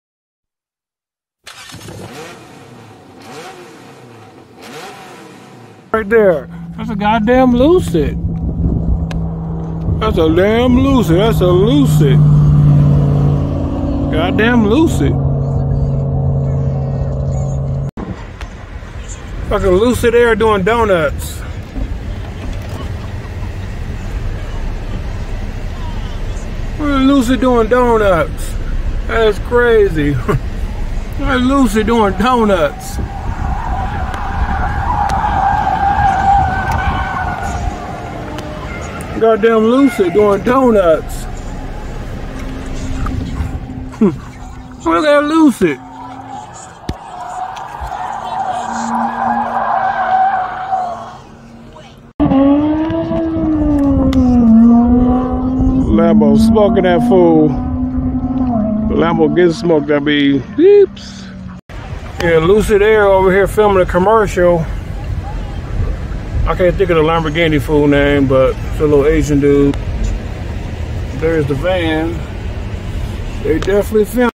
LUCID AIR DREAM DRIFTING vs ALEX CHOI LAMBORGHINI
Was fliming and seen alexchoi dragracing a lucid pretty cool

 Also check my Instagram out